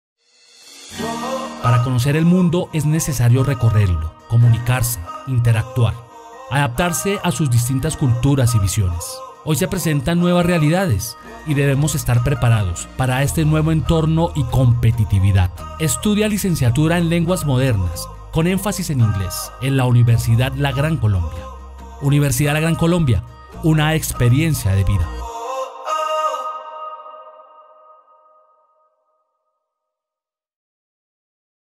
¡Estudia Licenciatura en Lenguas Modernas con énfasis en inglés en la UGC!
La nueva realidad nos impone asumir retos importantes y uno de ellos es la internacionalización. Atrévete a conocer, comprender y evaluar los saberes socioculturales que nacen a partir de la expresión de las lenguas maternas y modernas en el mercado global.